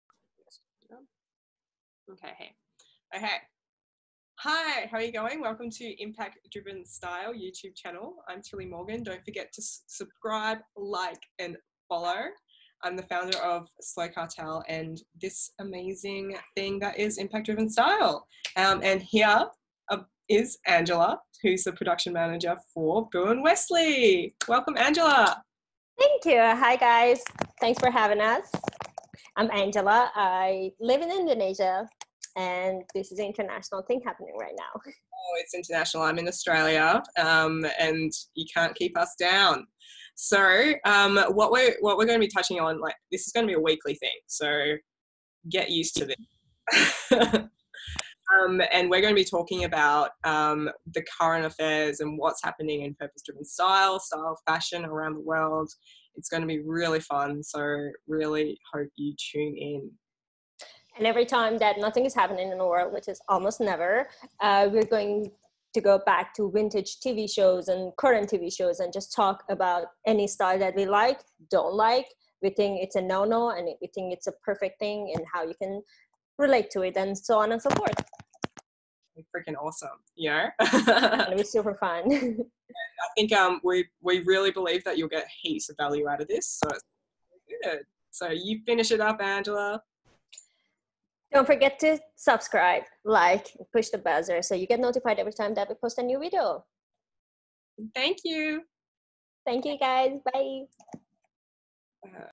INTRO Tillie & Angela's Weekly take on Impact Driven Style
Introducing you to the world of Tillie & Angela.
Tillie is the Founder & Director of Slow Cartel // Personal & Brand Stylist at Impact Driven Style.
Angela is Product & Innovation Manager at Boo & Westley.
Each week we will be taking on current affairs in the purpose-driven and sustainable fashion world. Bringing you fun and interesting content and takes as we work our way through and grow in this amazing industry.

Gonna be a ride Ladies and Gents xx